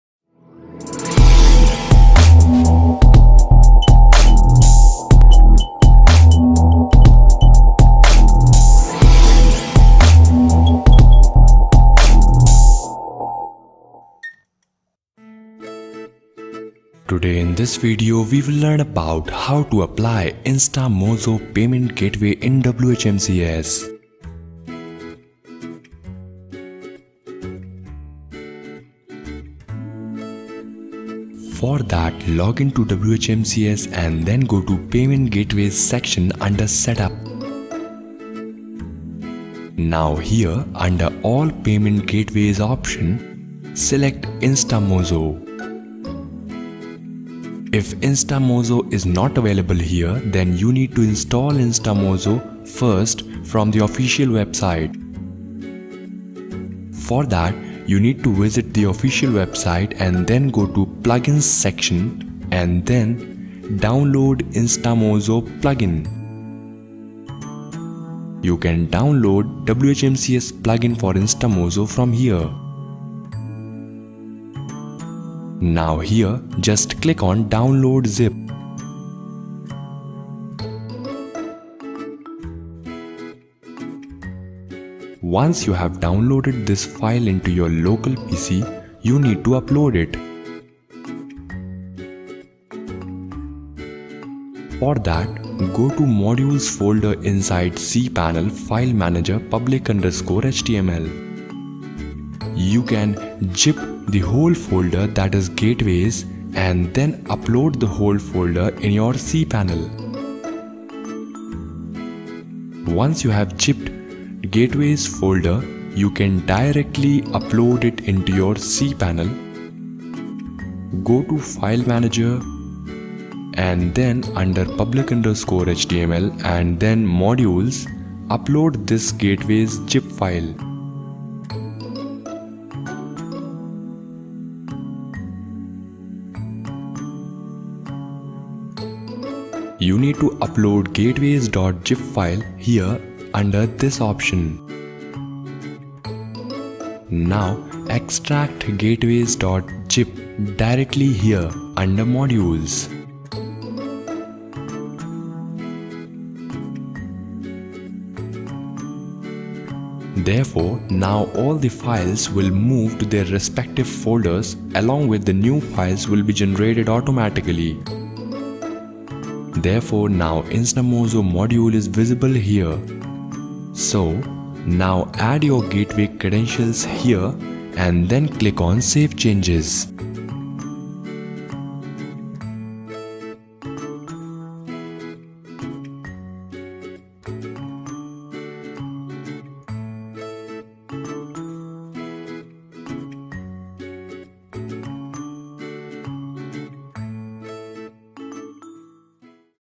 HOW TO ADD INSTAMOJO PAYMENT GATEWAY IN WHMCS? [STEP BY STEP]☑️
Today in this video you will learn how to add Instamojo payment gateway in your WHMCS.
Follow these simple steps to add Instamojo gateway in WHMCS:

Step::1 Login to your WHMCS Interface and go to Payment Gateway option under Payment section in Setup Tab.
Step::2 Check if Instamojo is available or not..If not proceed ahead
Step::4 Scroll till the bottom of the page and look for All Plugins Option..Click on it!!!
Step::5 Now Click on the Download icon on WHMCS logo

or else direct click on the link given below-

Step::6 A zip file will be downloaded in your local PC
Step::7 Find the zip file and extract the files
You will get Instamojo.php and response.php file after extracting the zip file
Step::8 Now you have to upload these two files on your server
Step::9 To do this Login to cPanel
Step::10 Go to the path where you installed WHMCS
Step::11 Then go to modules/gateways and upload both the files inside Gateway directory
Step::12 After uploading these files you will see Instamojo Gateway option added in your WHMCS
Step::13 Now add the Gateway credentials and click on save changes.
Step::14 That's it!!!

Follow the methods mentioned in this video to configure Instamojo payment gateway in your WHMCS. Take care of every step so that you don’t miss anything. If you have any query, feel free to let us know via the comments section below.